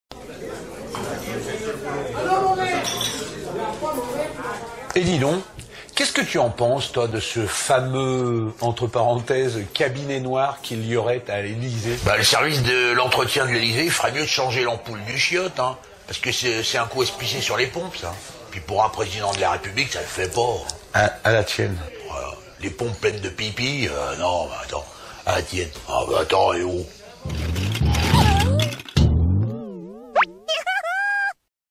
Brèves de comptoir - Le Cabinet noir de l'Elysée
On parle du fameux CABINET NOIR de l'Elysée dénoncé par le candidat "Les Républicains", François Fillon,  au bistro aujourd'hui.